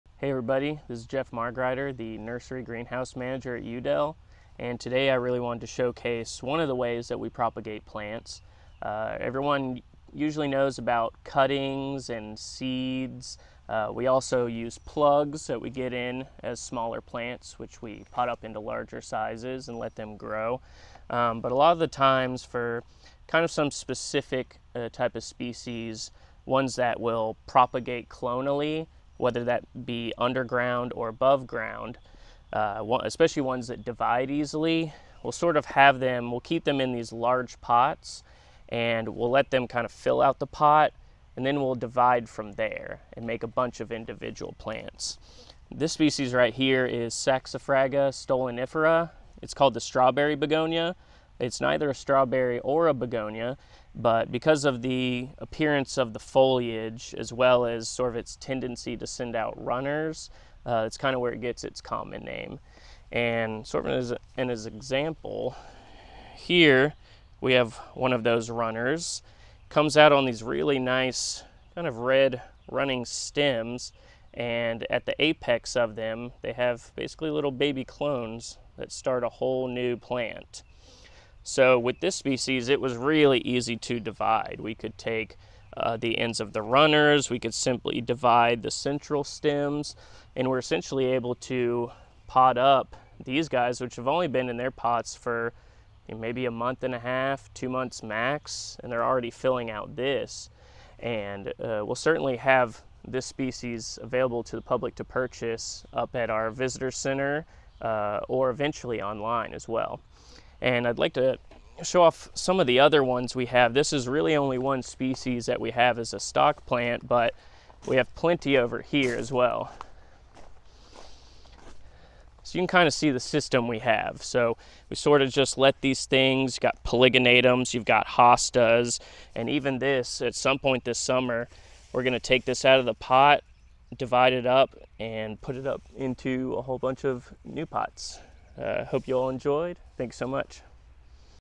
Propagating at Yew Dell Nursery
Yew Dell Botanical Gardens Nursery & Greenhouse Manager Jeffrey
Margreiter explains how within the nursery, we have an impressive collection of fascinating plants that we maintain in large pots as “stock plants.” Many species that originate as a single plant will, over time, expand. Sometimes it’s underground (rhizomes, tubers, or bulbs). Other times they will expand aboveground via stolons or create clonal bulblets on their foliage. Either way, that original plant will fill up the pot with a great number of clones that we then divide and move into many individual pots. Take the 𝘚𝘢𝘹𝘪𝘧𝘳𝘢𝘨𝘢 𝘴𝘵𝘰𝘭𝘰𝘯𝘪𝘧𝘦𝘳𝘢, also known as the Strawberry Begonia (it is neither a strawberry or begonia by the way). Stunning red stolons expand out from the center of the plant, appearing as long, leafless stems. But at the tip of each of these stolons, there is a miniature, brand-new clone which finds an uncrowded spot of soil to take root. After dividing out a few hundred of these smaller clones, we inserted them into plug flats with aerial root-pruning bottoms to help stimulate healthy root systems. Once they fill out their individual cells above and below ground, these plugs are then potted up into larger pots of their own and will soon be available for purchase. What originated as a single large pot has now been divided into an abundance of individual Strawberry Begonias- each with the capability to start a whole new “stock plant!”